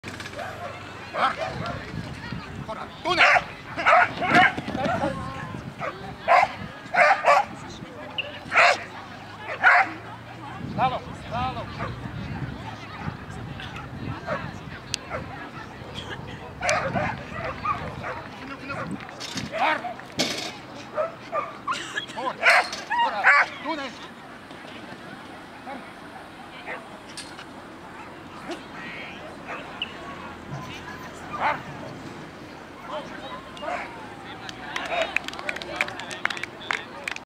1815.ch - Agility Meeting Steg 2015
Hunde und ihre Halter unterwegs auf anspruchsvollen Parcours.